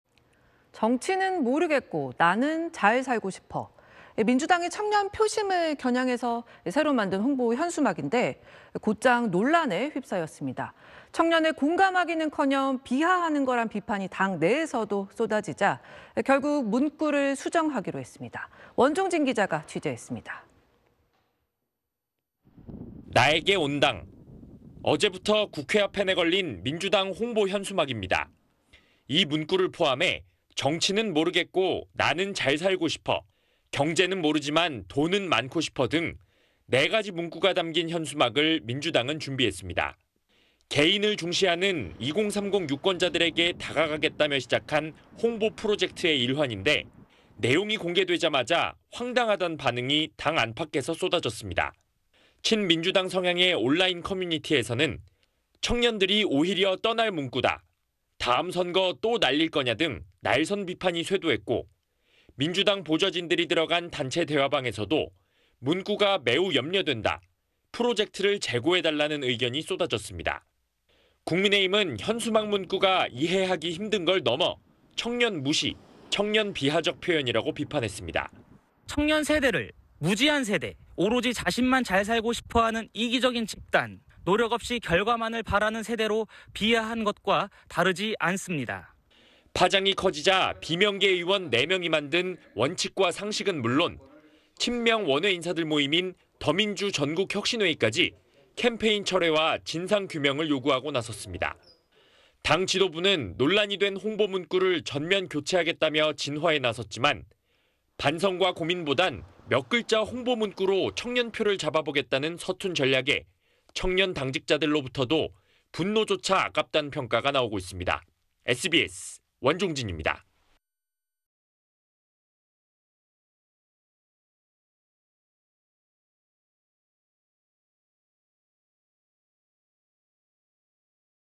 "정치는 모르겠고…" 민주당 청년 겨냥 현수막 '비하' 논란 / SBS 8뉴스
〈앵커〉

'정치는 모르겠고, 나는 잘 살고 싶어.' 민주당이 청년 표심을 겨냥해 새로 만든 홍보 현수막인데 곧장 논란에 휩싸였습니다. 청년을 공감하기는커녕 비하하는 거라는 비판이 당 내에서도 쏟아지자, 결국 문구를 수정하기로 했습니다.

원종진 기자가 취재했습니다.

〈기자〉

'나에게 온당.'

어제(17일)부터 국회 앞에 내걸린 민주당 홍보 현수막입니다.

이 문구를 포함해 '정치는 모르겠고 나는 잘살고 싶어', '경제는 모르지만 돈은 많고 싶어' 등 4가지 문구가 담긴 현수막을 민주당은 준비했습니다.

'개인'을 중시하는 20·30 유권자들에게 다가가겠다며 시작한 홍보 프로젝트의 일환인데, 내용이 공개되자마자 황당하다는 반응이 당 안팎에서 쏟아졌습니다.

친 민주당 성향의 온라인 커뮤니티에서는 청년들이 오히려 떠날 문구다, 다음 선거 또 날릴 거냐 등 날 선 비판이 쇄도했고, 민주당 보좌진들이 들어간 단체 대화방에서도 문구가 매우 염려된다, 프로젝트를 재고해 달라는 의견이 쏟아졌습니다.

국민의힘은 현수막 문구가 이해하기 힘든 것을 넘어 청년 무시, 청년 비하적 표현이라고 비판했습니다.

[신주호/국민의힘 부대변인 : 청년 세대를 무지한 세대, 오로지 자신만 잘 살고 싶어 하는 이기적인 집단, 노력 없이 결과만을 바라는 세대로 비하한 것과 다르지 않습니다.]

파장이 커지자 비명계 의원 4명이 만든 '원칙과 상식'은 물론, 친명 원외 인사들 모임인 '더민주 전국혁신회의'까지 캠페인 철회와 진상규명을 요구하고 나섰습니다.

당 지도부는 논란이 된 홍보 문구를 전면 교체하겠다며 진화에 나섰지만, 반성과 고민보다는 몇 글자 홍보 문구로 청년 표를 잡아보겠다는 서툰 전략에 청년 당직자들로부터도 '분노조차 아깝다'는 평가가 나오고 있습니다.

(영상취재 : 양두원, 영상편집 : 유미라)